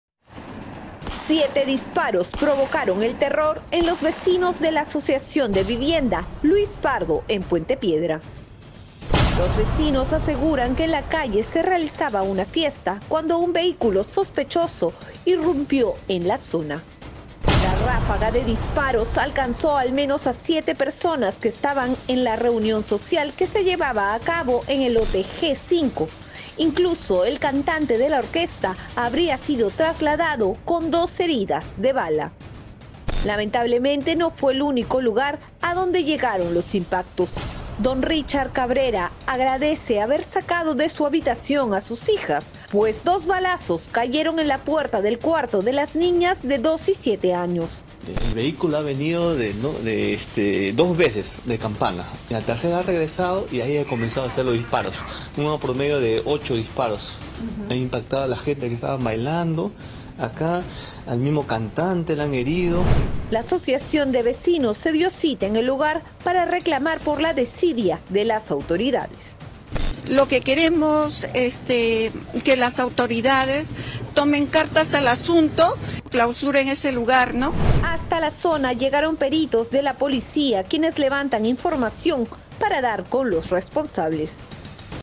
Puente Piedra: Vecinos festejaban en la calle y terminan baleados por unos delincuentes
El hecho ocurrió, específicamente, en la Asociación de Vivienda Luis Pardo, Puente Piedra.